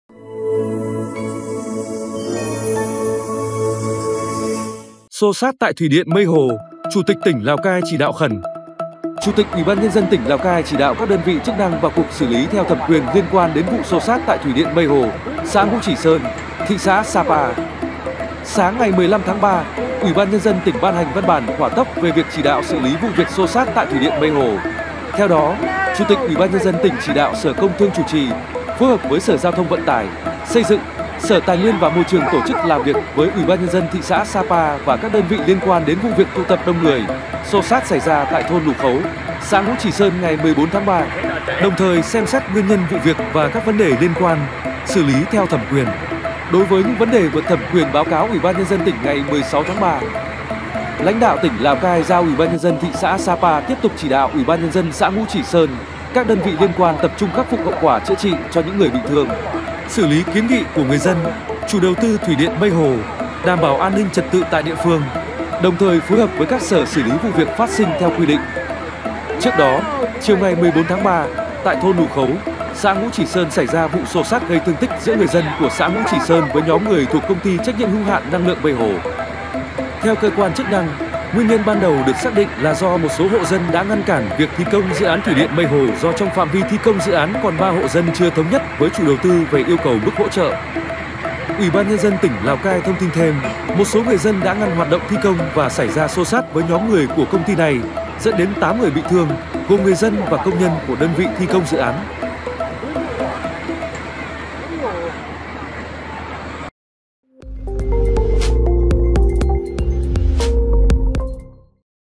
Xô xát tại thủy điện Mây Hồ: Chủ tịch tỉnh Lào Cai chỉ đạo khẩn | Thien Minh Holdings
Sáng ngày 15/3, UBND tỉnh ban hành văn bản hỏa tốc về việc chỉ đạo xử lý vụ việc xô xát tại thủy điện Mây Hồ.
Theo đó, Chủ tịch UBND tỉnh chỉ đạo sở Công Thương chủ trì, phối hợp với Sở GTVT- Xây dựng, Sở TN&MT tổ chức làm việc với UBND thị xã Sa Pa và các đơn vị liên quan đến vụ việc tụ tập đông người, xô xát xảy ra tại thôn Lủ Khấu, xã Ngũ Chỉ Sơn ngày 14/3. Đồng thời xem xét nguyên nhân vụ việc và các vấn đề liên quan, xử lý theo thẩm quyền. Đối với những vấn đề vượt thẩm quyền báo cáo UBND tỉnh ngày 16/3.
Lãnh đạo tỉnh Lào Cai giao UBND thị xã Sa Pa tiếp tục chỉ đạo UBND xã Ngũ Chỉ Sơn, các đơn vị liên quan tập trung khắc phục hậu quả chữa trị cho những người bị thương. Xử lý kiến nghị của người dân, chủ đầu tư thủy điện Mây Hồ, đảm bảo an ninh trật tự tại địa phương. Đồng thời phối hợp với các Sở xử lý vụ việc phát sinh theo quy định.